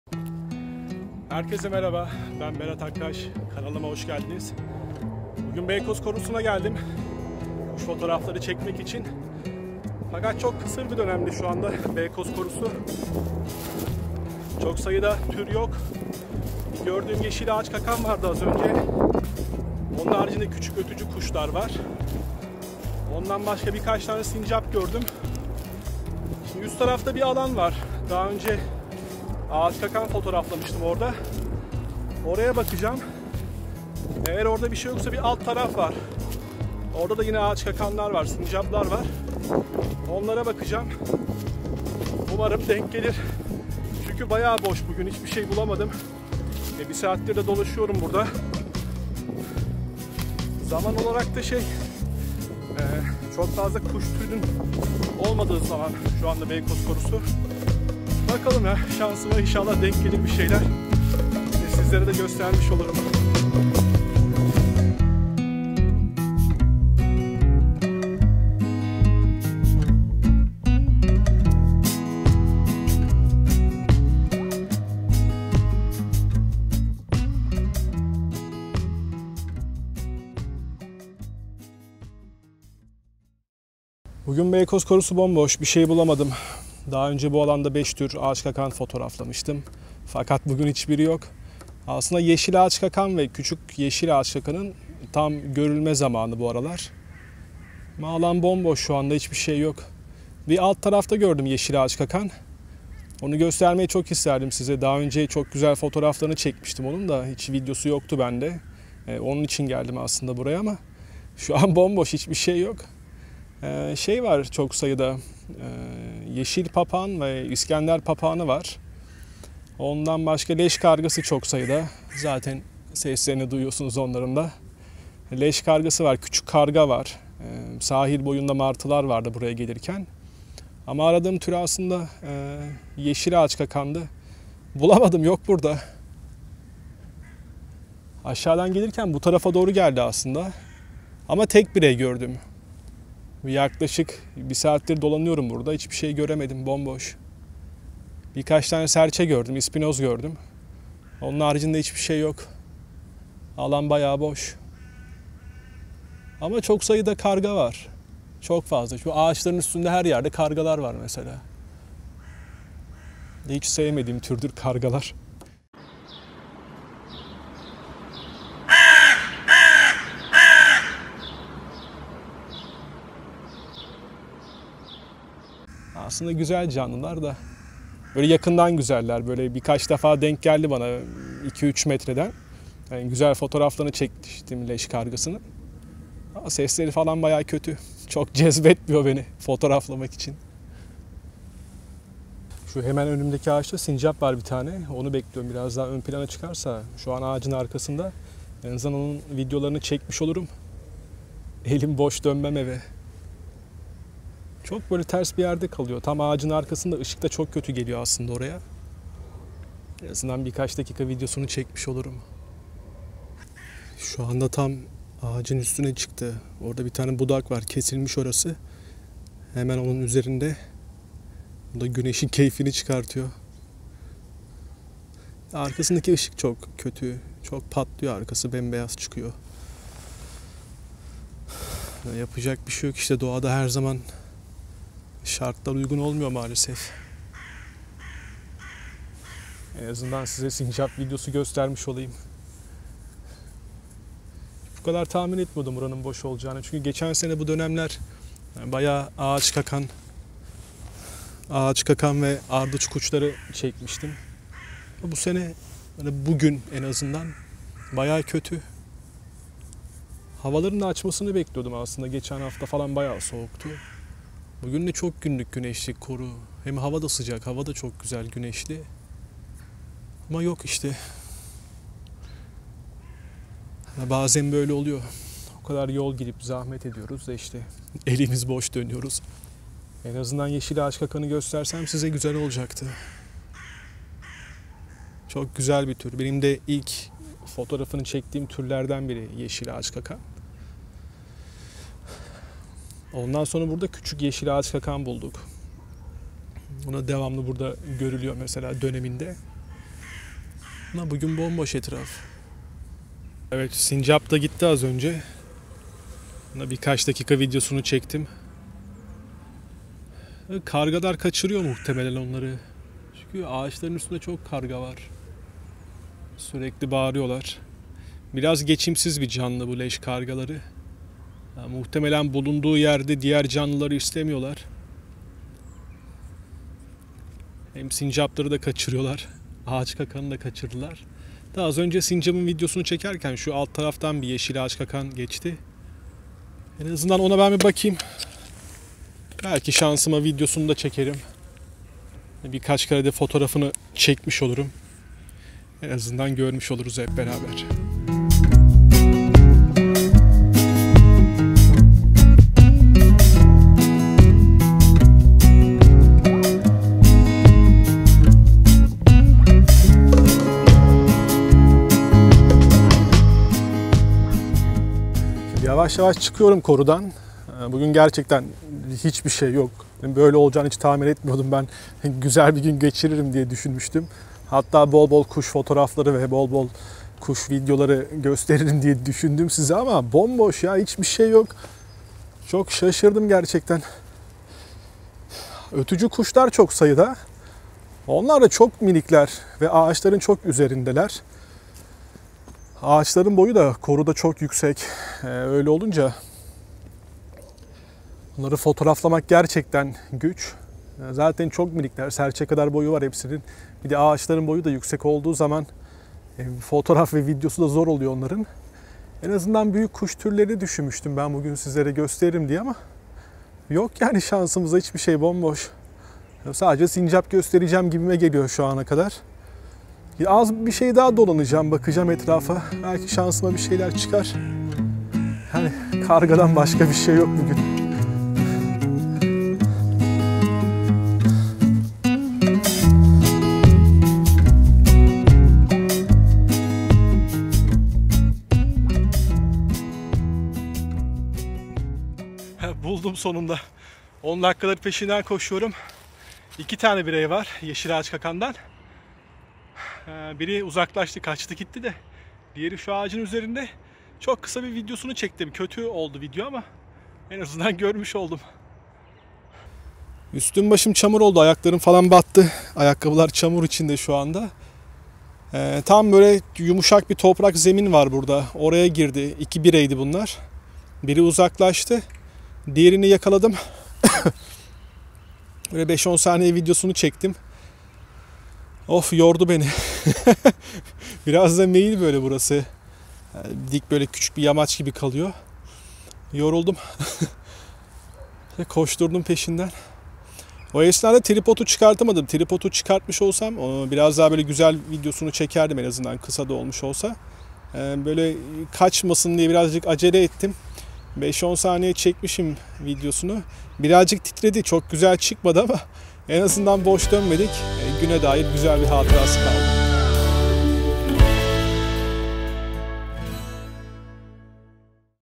Siz Hiç Yeşil ''Ağaçkakan'' Gördünüz Mü ?
Merhaba ben Berat Akkaş
Yaklaşık 6 yıldır Kuş Fotoğrafçılığı yapıyorum.
Beykoz Korusu ise benim favori kuş fotoğraflama alanlarımdan birisi. Yıl içerisinde bir çok türe ev sahipliği yapıyor. Alanda 5 tür ağaçkakan yaşamakta. Bir çok ötücü kuşa ve Alaca Baykuş gibi güzel bir türe de ev sahipliği yapmaktadır.

Kuş Fotoğrafçıları ve Poyrazköy mini belgeseli için linki tıklayabilirsiniz.